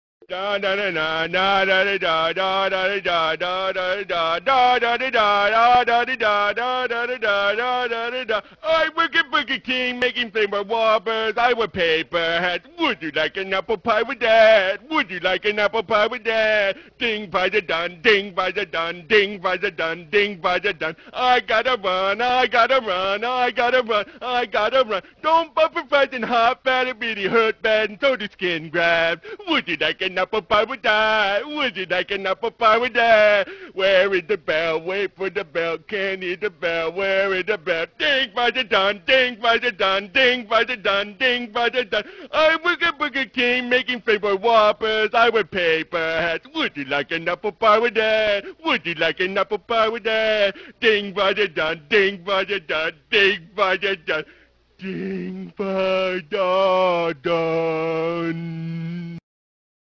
LAWTIDE1X SINGS A X-MAS CAROL
LAWTIDE1X sings us a Christmas classic! Grab your egg nog or malt liquor and enjoy!